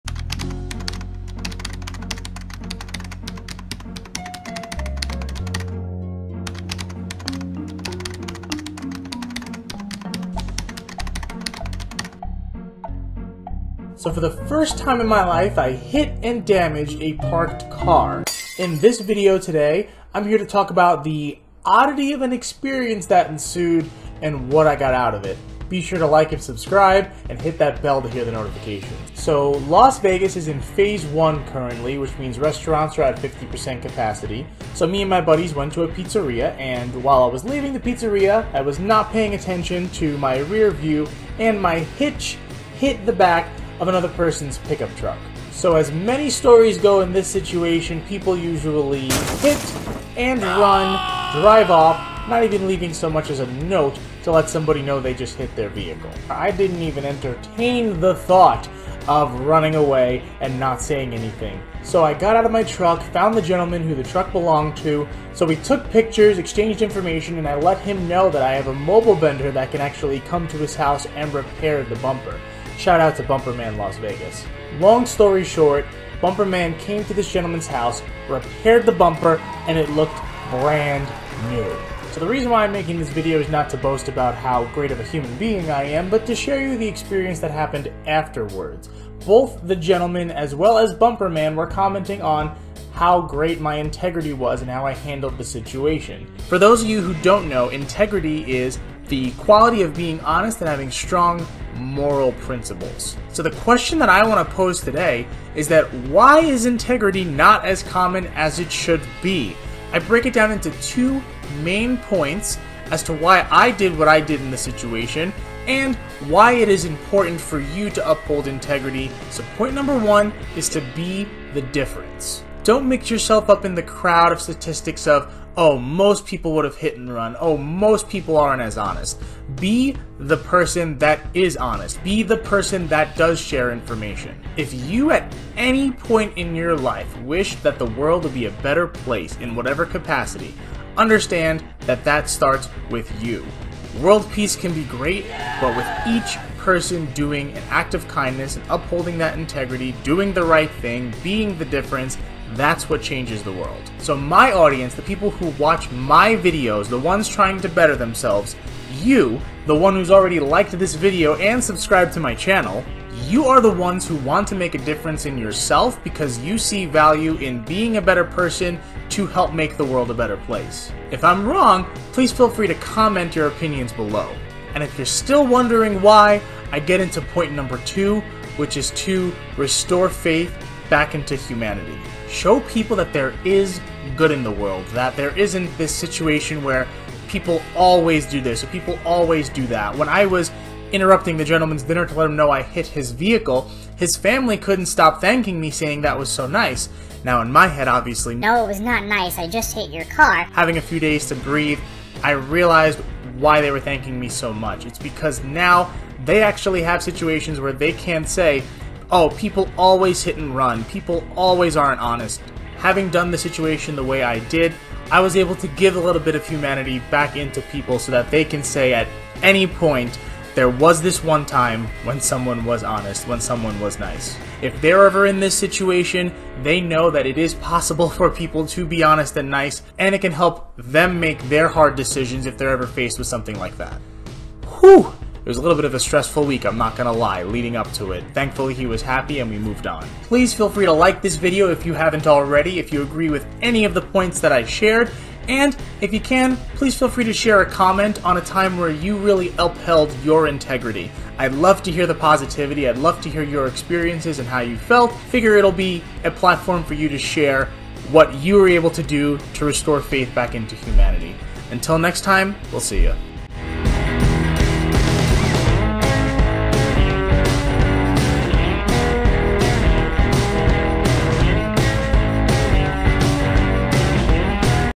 WHY YOU SHOULD HAVE INTEGRITY: The Importance of Integrity And Why It's Important
I HIT A CAR and decided to share this lesson on the importance of Integrity... and paying attention while you're driving.

WHY am I telling you this story? Because this situation, and other situations involving honesty and “doing the right thing” are not Common.

WHY SHOULD HAVING INTEGRITY BE COMMON?

TWO POINTS:
1) Be the Difference.
MY audience, the people who watch my content, YOU are people who want to better yourselves and YOU want to be the difference. The change "for the better" we want to see in the world starts with YOU.

2) Give others Faith in humanity.
- Show people that there is good in the world and that people who have integrity, people who do the right thing, exist. Faith goes a long way.

- For other helpful hints, guides or to talk about your dreams.

 "Everyone has a purpose to be the best version of themselves they can be."
- Keith E. Fiandra

Thank you for your support! It is my privilege to provide you with free content every week!